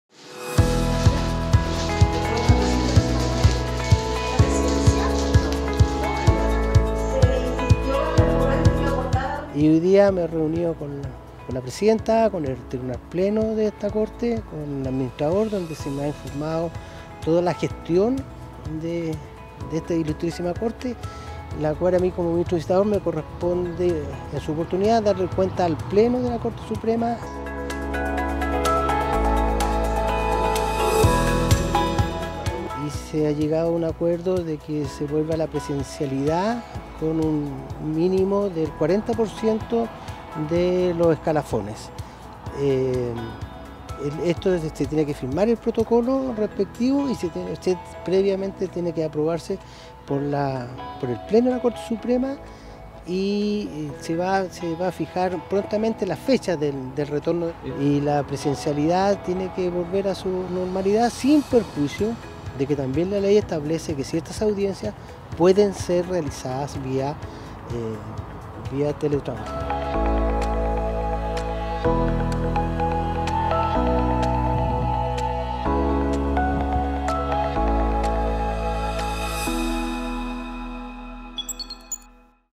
Ministro de la Corte Suprema Manuel Valderrama se reúne con pleno de la Corte de Punta Arenas
En la primera inspección que se realiza en forma presencial desde 2019, el ministro de la Corte Suprema Manuel Antonio Valderrama se constituyó el lunes 3 de octubre a primera hora de la mañana en la Corte de Apelaciones de Punta Arenas, donde fue recibido por los ministros Marcos Kusanovic, Caroline Turner y Claudio Jara (s) y el fiscal judicial, Pablo Miño, encabezados por su presidenta, María Isabel San Martín.